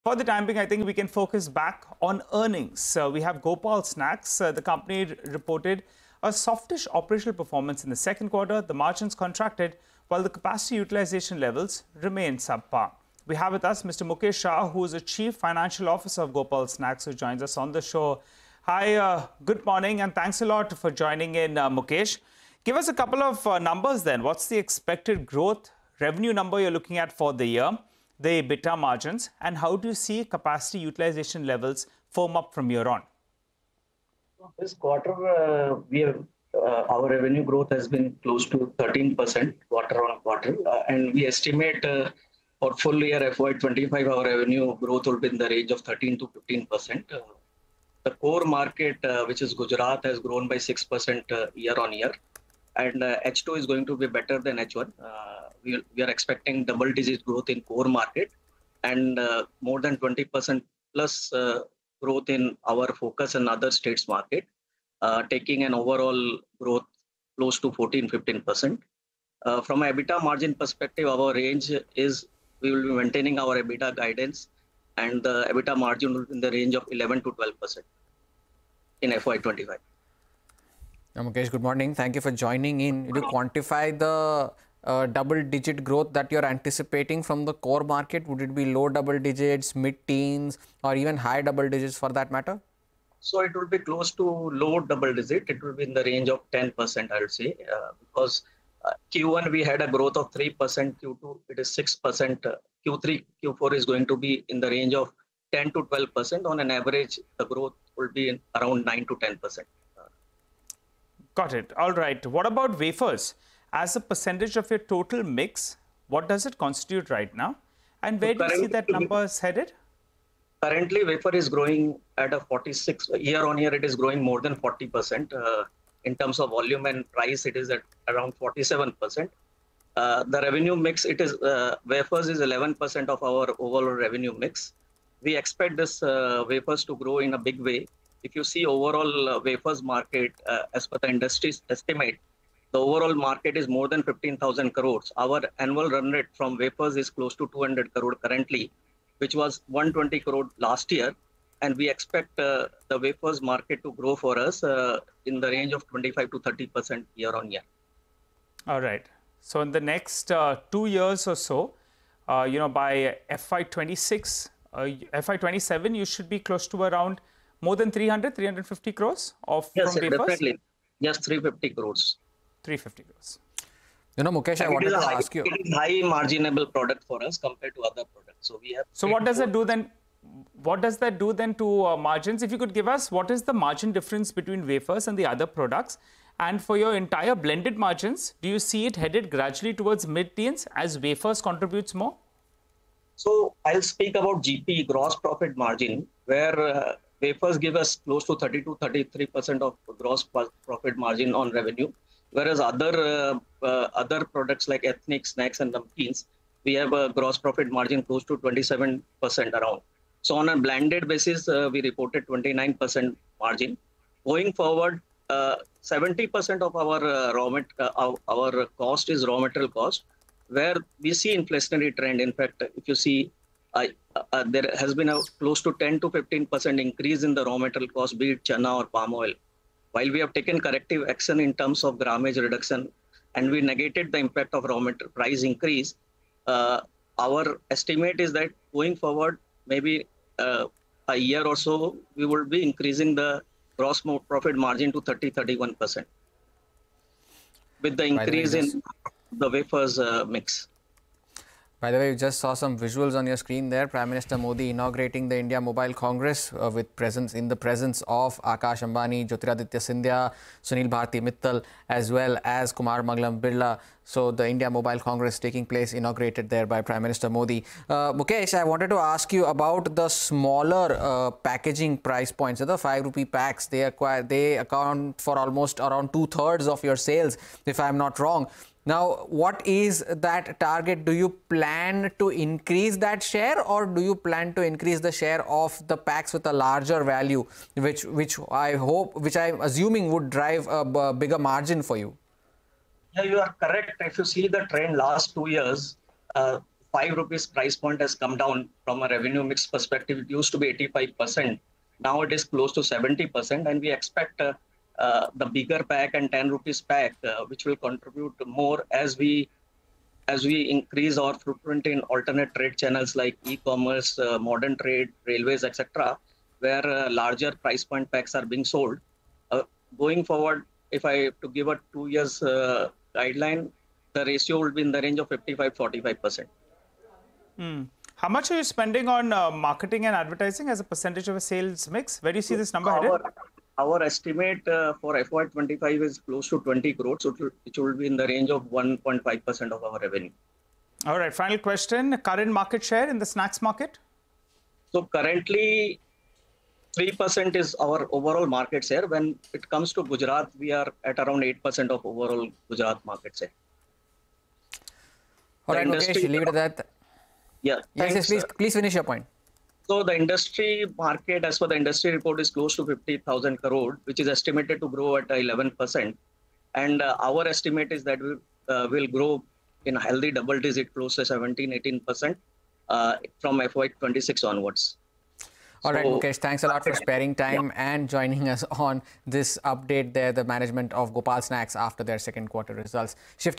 Expect Gross Profit Margin To Rise To 30-31%, With Increase Of Wafers In Overall Mix: Gopal Snacks
2QWithCNBCTV18 | Expect low double-digit growth in range of 10% says Mukesh Shah, CFO of Gopal Snacks. He tells Hormaz Fatakia and Nigel D'Souza that company expects gross profit margin to increase to 30-31%,with the increase of wafers in the overall mix.

n18oc_business